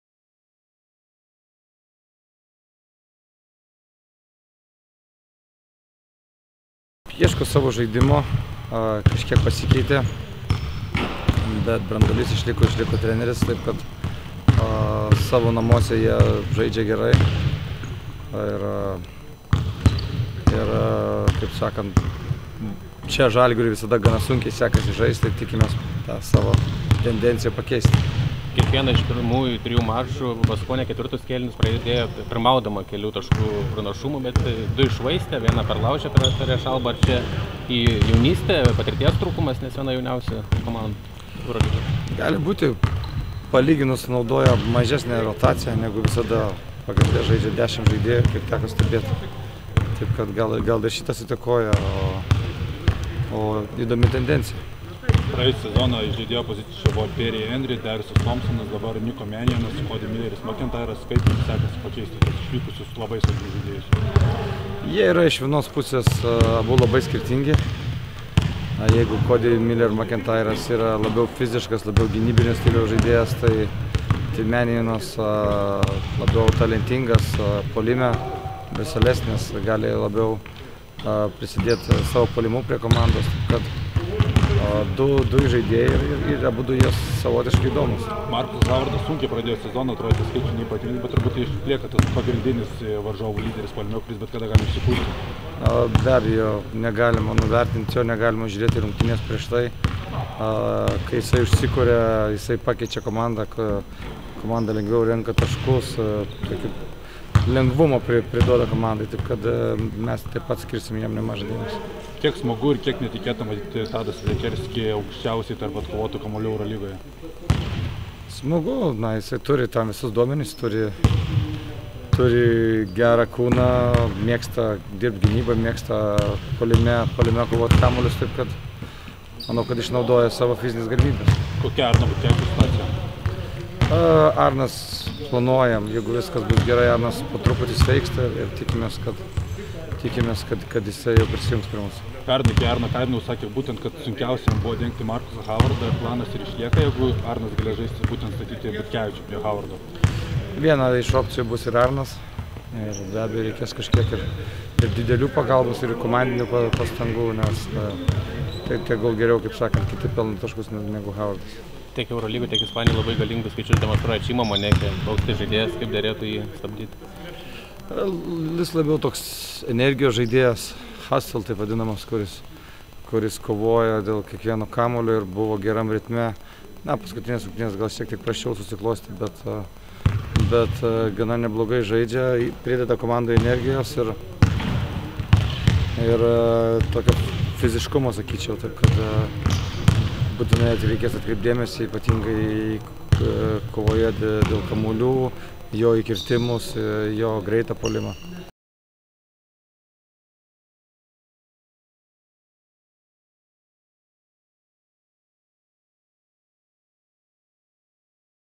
K.Maksvytis prieš dvikovą su „Baskonia“: „Tikimės, kad Arnas jau prisijungs prie mūsų“
Kauno „Žalgirio“ vyriausiasis treneris Kazys Maksvytis bendravo su žiniasklaida Vitorijoje rungtynių su vietos „Baskonia“ išvakarėse.